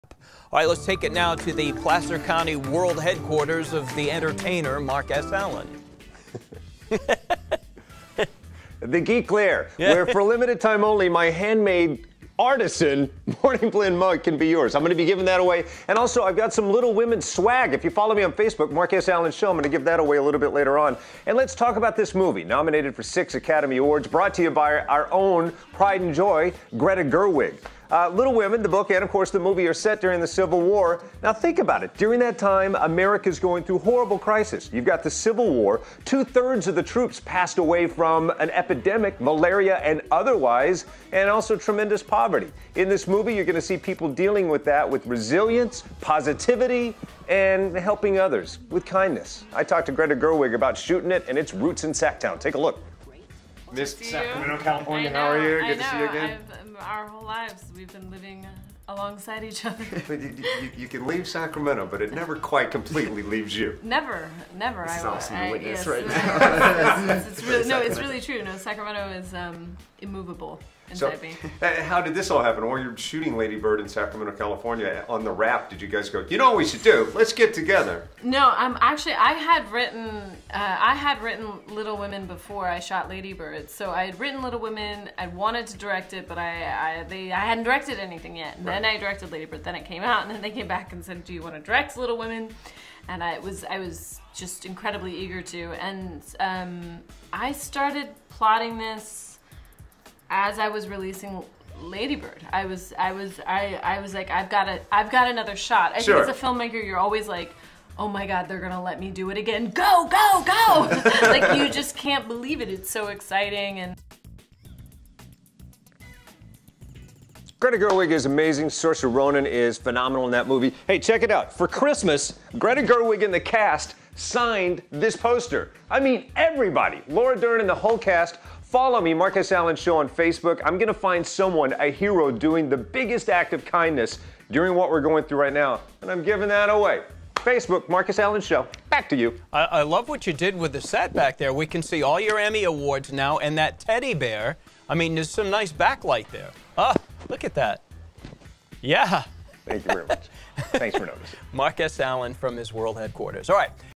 'Little Women' from Sacramento's own Greta Gerwig now available to rent
Follow Mark S. Allen on Facebook to win some Little Women swag, or just rent the movie now from the comfort of your home.